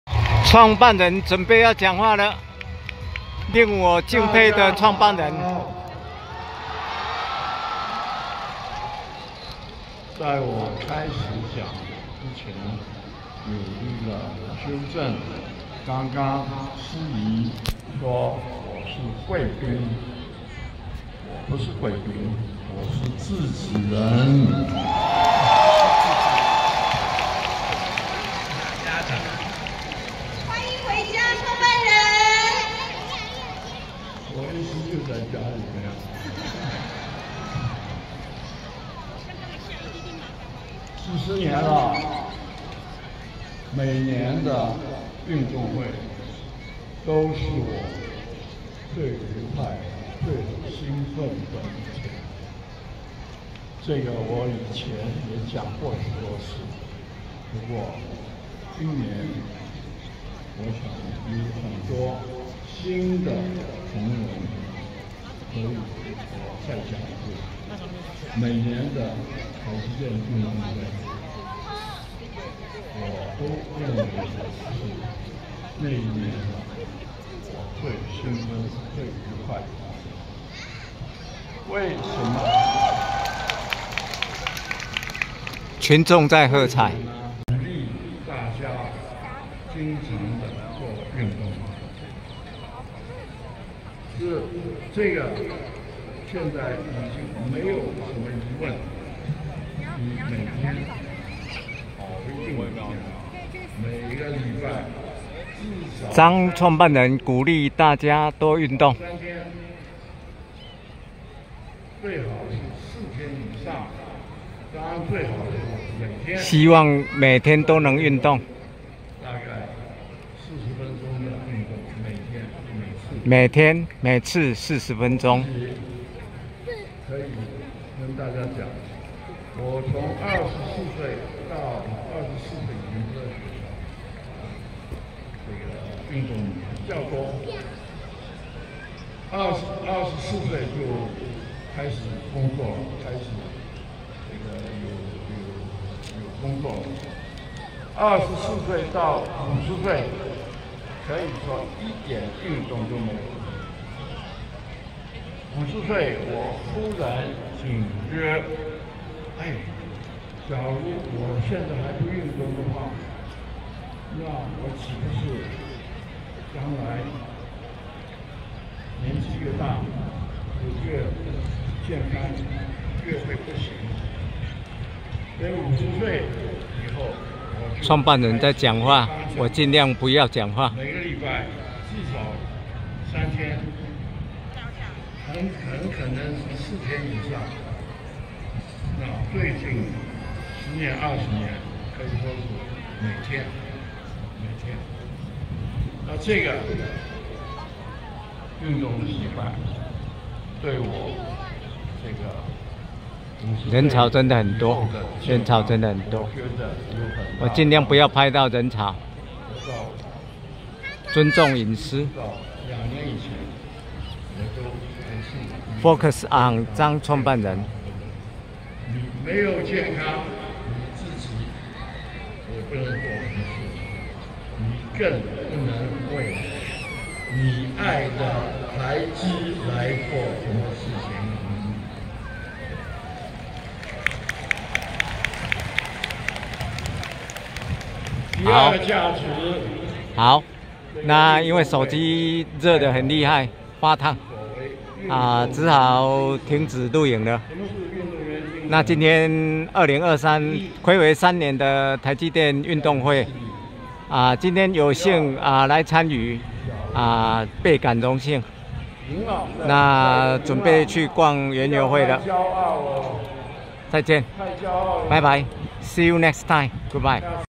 226運動奈米--2023台積電運動會張忠謀創辦人談話:首重運動與健康（Morris Chang:Key remind is exercise and health, Taiwan）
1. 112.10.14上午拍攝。